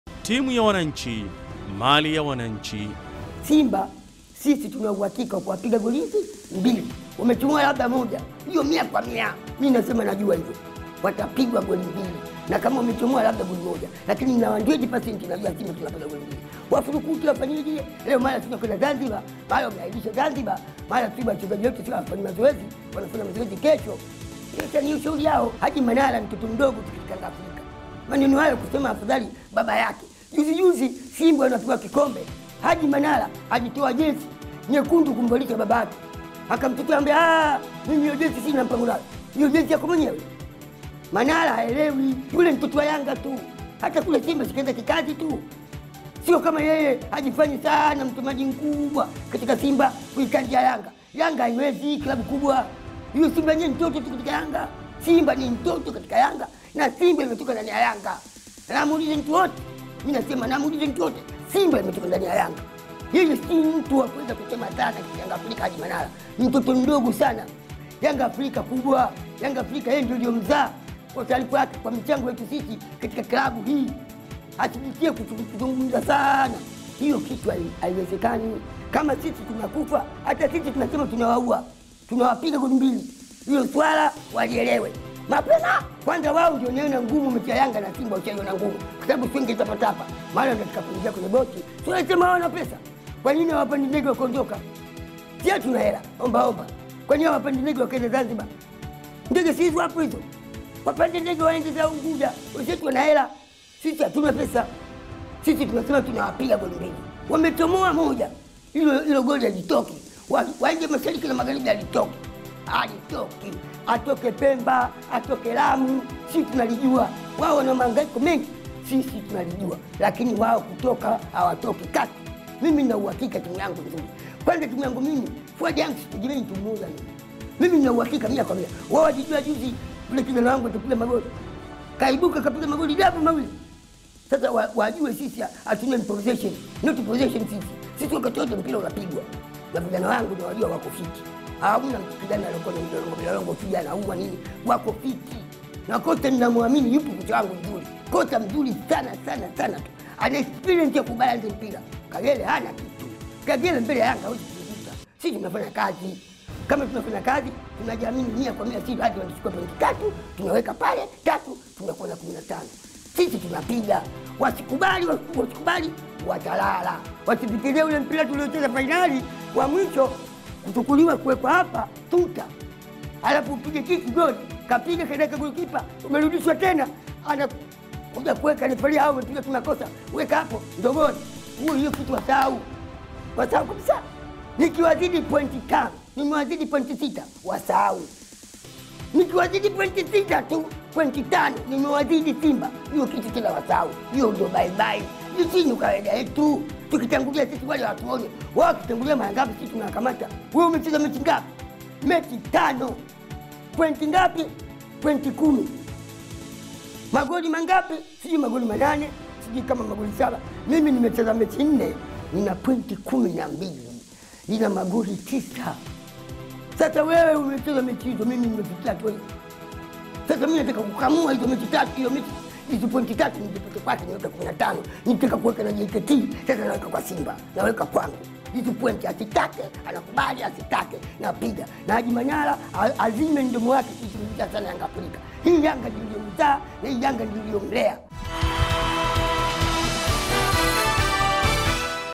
KUELEKEA SIMBA VS YANGA: Mzee wa Yanga amvaa Haji Manara, asema Simba itapigwa 2-0 za uhakika
TAMBO ZA MASHABIKI: Mzee Haji Omari ni mwanachama wa Yanga, anasema kesho lazima Simbwa wapigwe mabao 2-0 au 2-1, huku akimshukia msemaji wa Simba, Haji Manara. Ni kuelekea mchezo wa Watani wa Jadi utakaopigwa kesho kwenye Uwanja wa taifa.